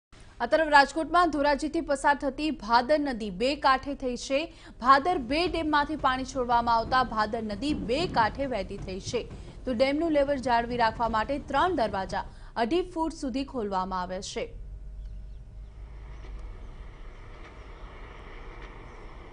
Rajkot: Water being released from Bhadar-2 dam into Bhadar river | TV9News
Rajkot: High inflow of water in Bhadar river | TV9News